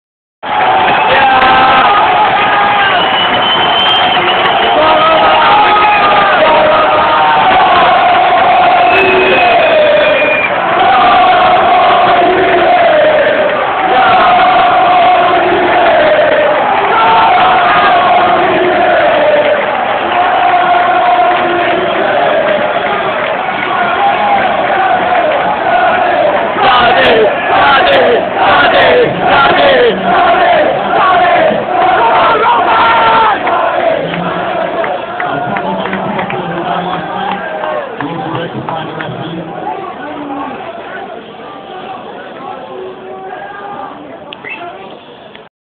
Dundee 1-2 Raith Rovers (Teams Coming Out)
The teams come out for the Quarter Final of the Scottish Cup.